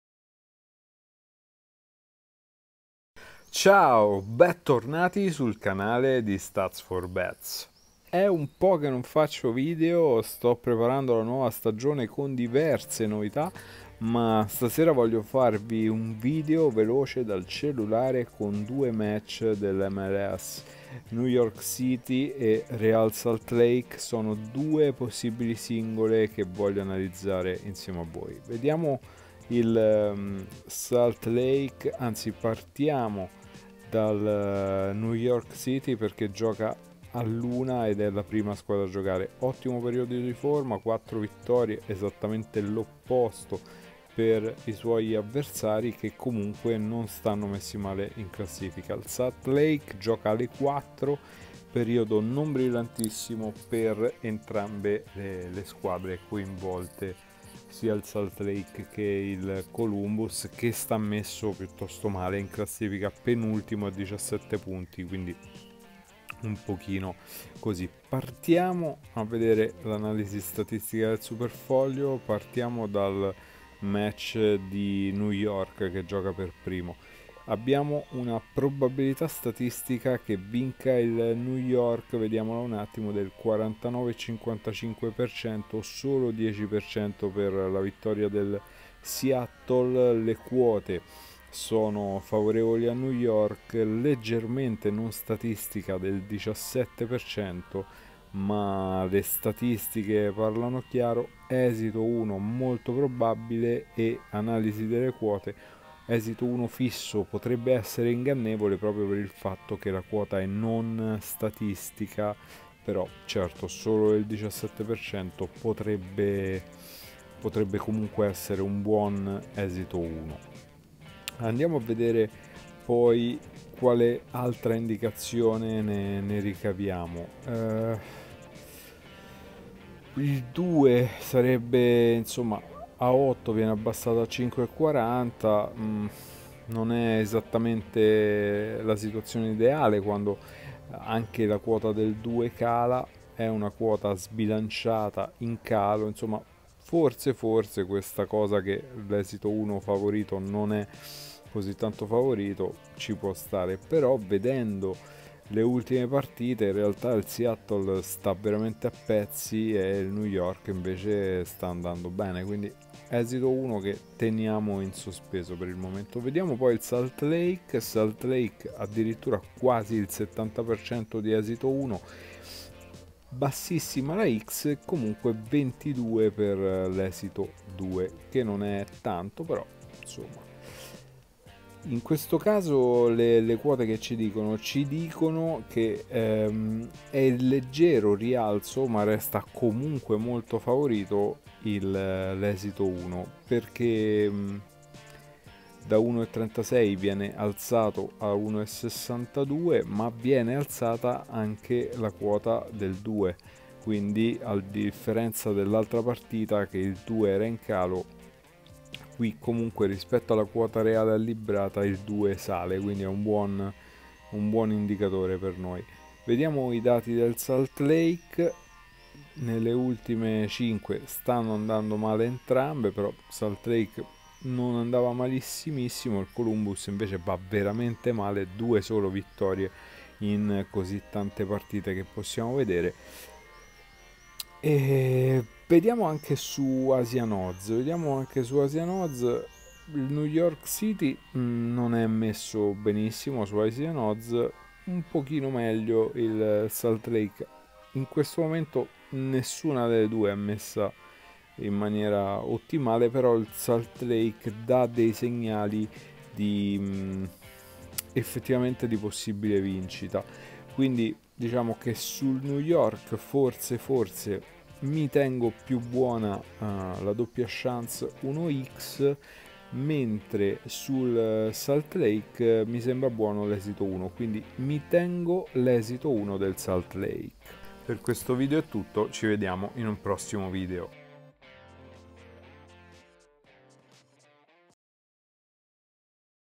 PRONOSTICI MLS - STUDIO CAMPIONATO AMERICANO CON SUPERFOGLIO 4.0
Stanotte giocano in MLS. Vediamo un paio di pronostici fatti con il SuperFoglio.